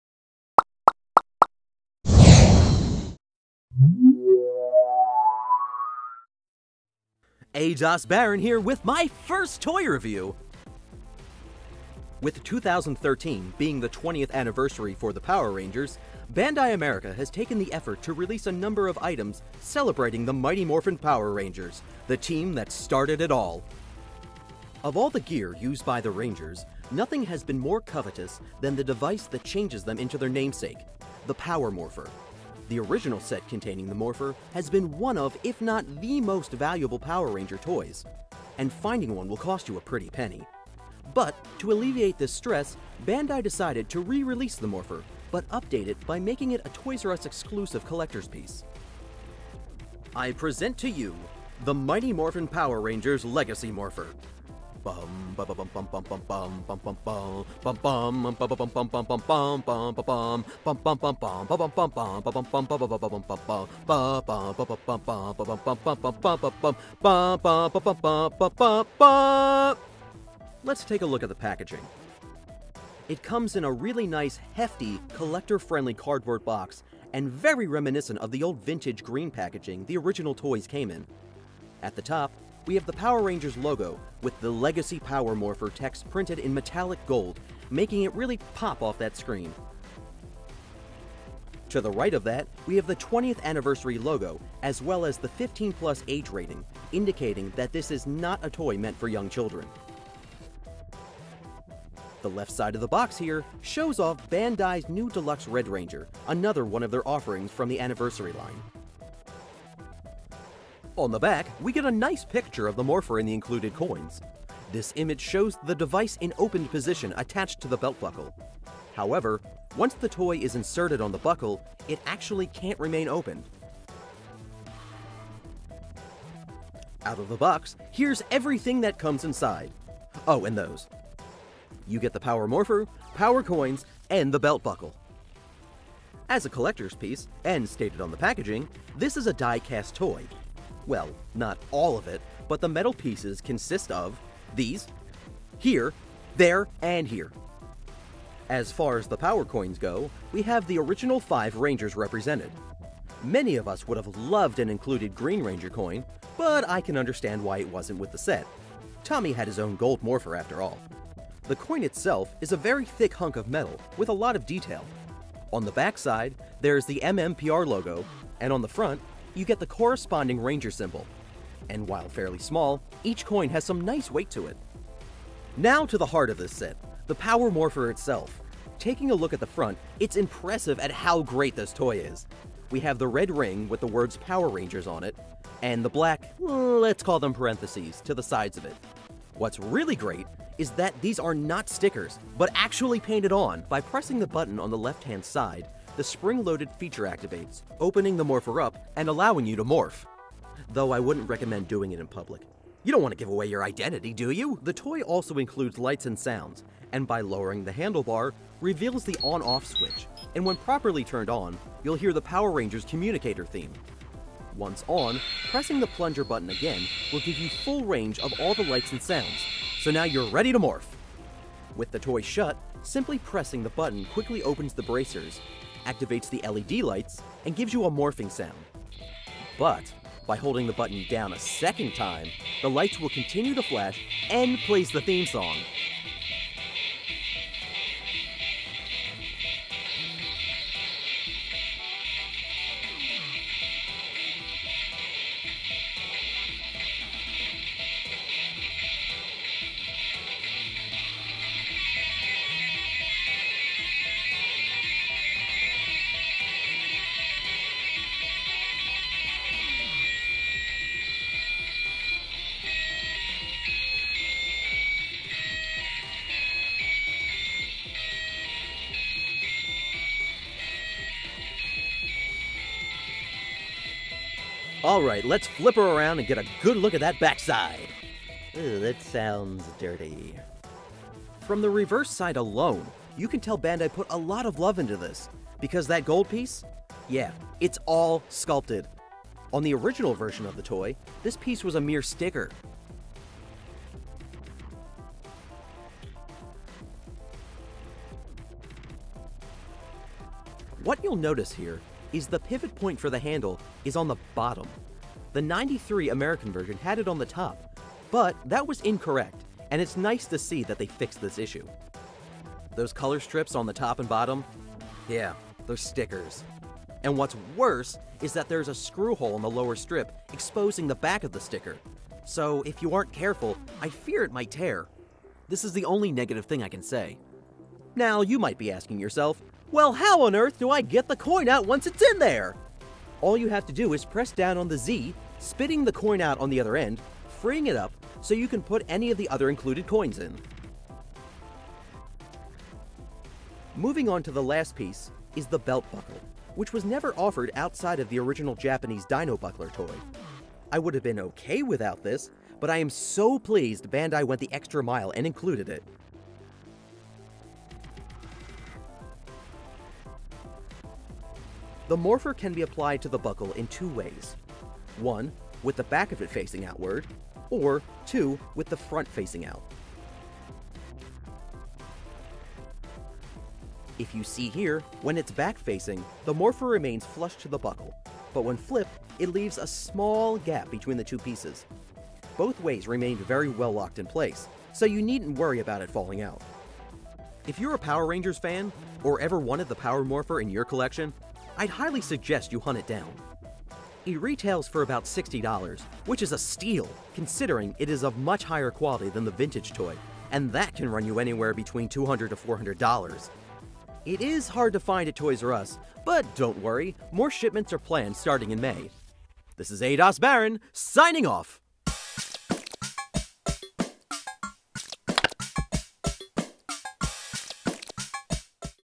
Mighty Morphin Power Rangers Legacy Power Morpher REVIEW
A. Das Baron gives the site his FIRST toy review, and to kick things off he gives us all an AMAZING look at the Mighty Morphing Power Rangers 20th Anniversary Toyapos;s R Us EXCLUSIVE Legacy Power Morpher!